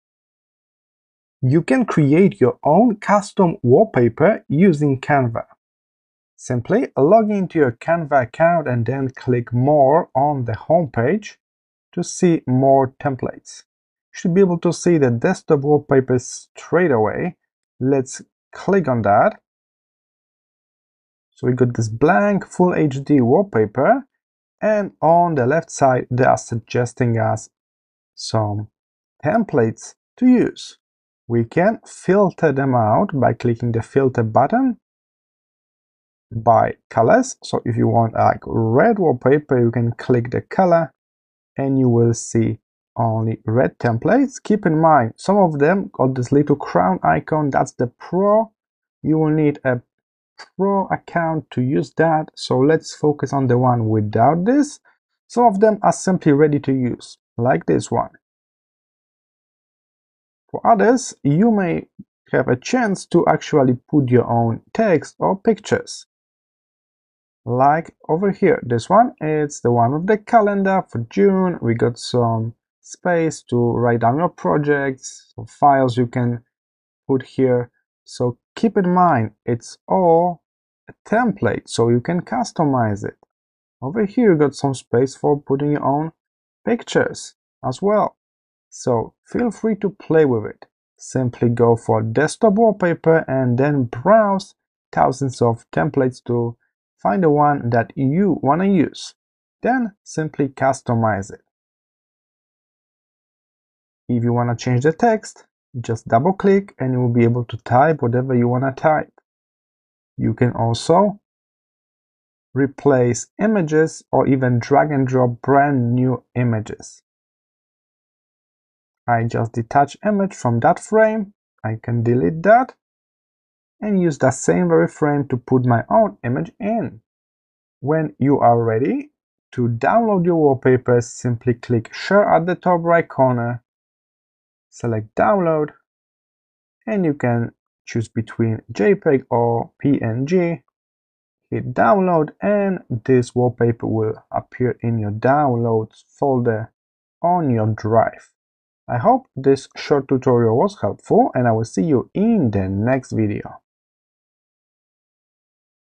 How to Make a Custom Wallpaper in Canva
Transform your device's screen with a custom wallpaper made in Canva! In this tutorial, we'll guide you through the process of creating a personalized wallpaper that reflects your style and personality. Learn how to choose the perfect dimensions for your device, explore Canva's extensive library of backgrounds and design elements, and add your own photos or graphics to make it truly unique. With Canva's user-friendly interface and versatile tools, designing a custom wallpaper is quick and easy. Watch now to learn how to customize your device with a stunning wallpaper created in Canva!